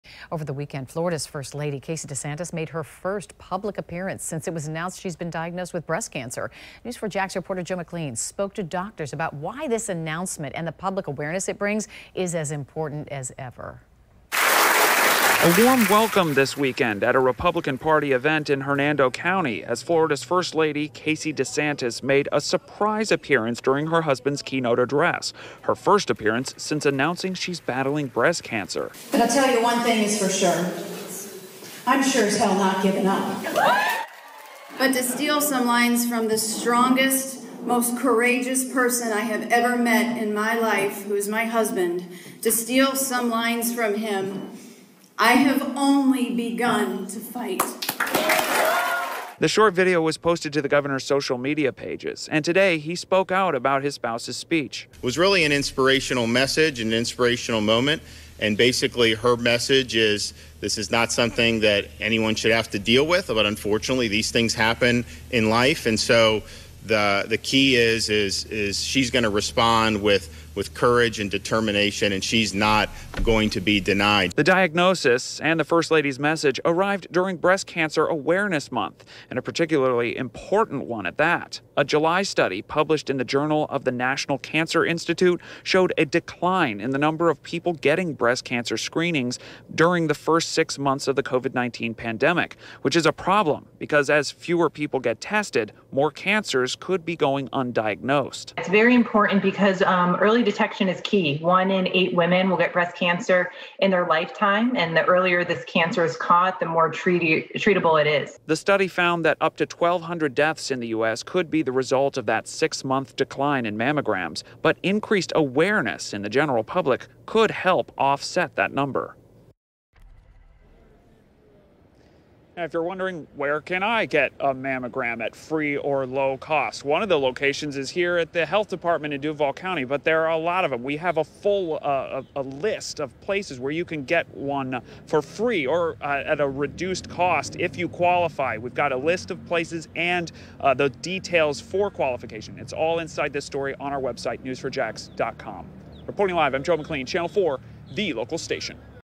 Casey DeSantis makes first public appearance since cancer diagnosis
Florida's First Lady Casey DeSantis made her first public appearance since it was announced she has been diagnosed with breast cancer. Oncologist , Jennifer Crozier says the public awareness is important.